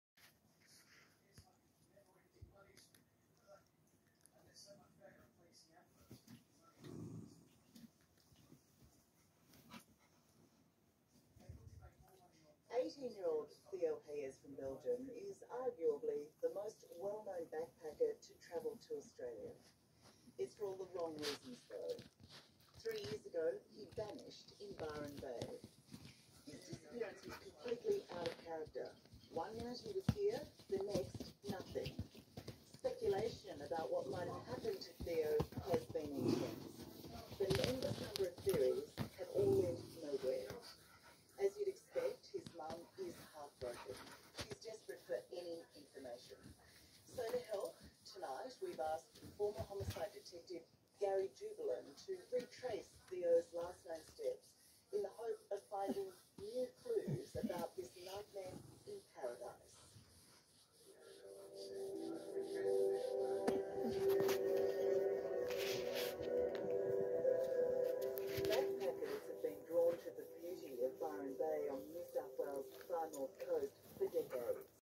Rainy days inside
Chilling inside together in bed on this rainy rainy day! ❤️😍🐶
Ivy the Maltipoo & her brother Beau the Pomeranian playing and hanging out w/ bestie Thor the Staffy ❤️😍🐶
Facebook.com/pupcakesdogcare